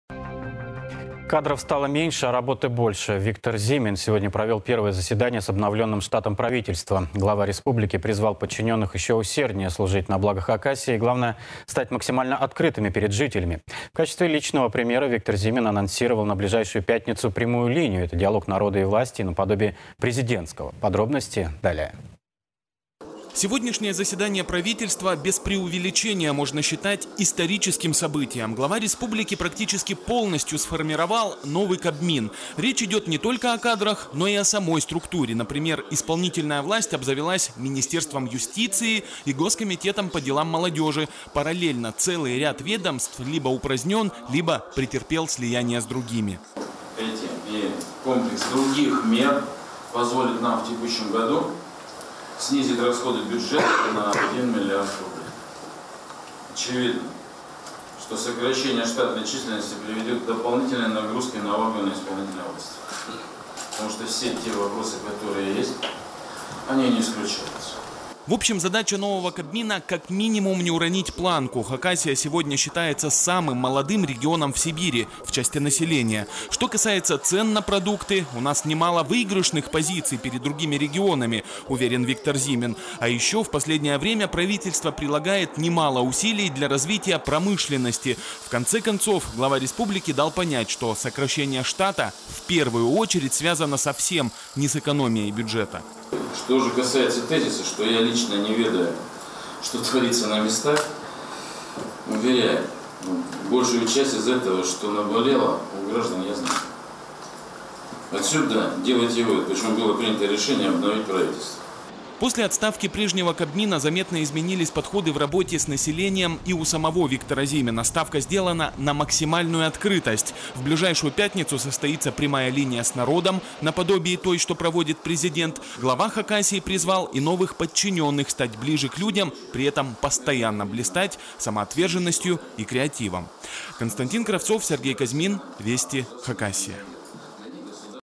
Обновленное правительство Хакасии, приступило к работе. Расскажем с чего решили начать. 18.07.2017
Кадров стало меньше, а работы больше. Виктор Зимин сегодня провел первое заседание с обновленным штатом Правительства. Глава республики призвал подчиненных еще усерднее служить на благо Хакасии и главное стать максимально открытыми перед жителями.  В качестве личного примера Виктор Зимин анонсировал на ближайшую пятницу Прямую линию - это диалог народа и власти на подобии Президентского.